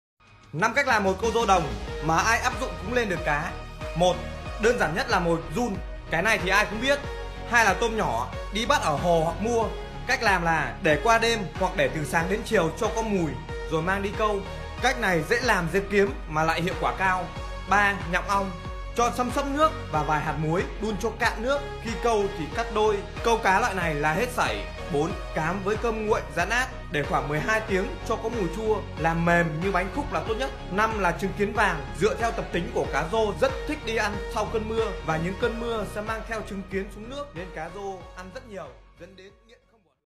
5 Cách Làm Mồi Câu Cá Rô Đồng 😄
5 cách làm mồi câu cá rô đồng
các bạn áp dụng thử nhé
1. câu bằng mồi giun
2. tôm nhỏ
3.sáp ong
4.cám và cơm nguội
5.trứng kiến vàng